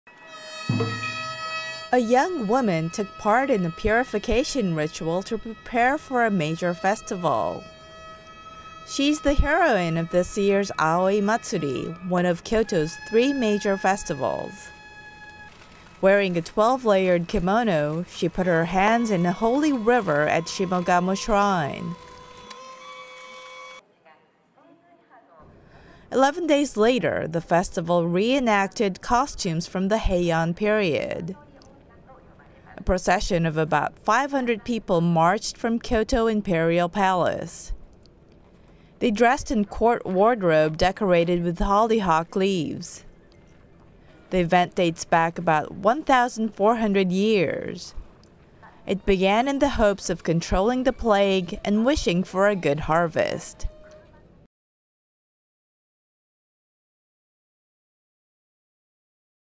Aoi festival colors Kyoto
The annual Aoi festival colors Japan's ancient capital.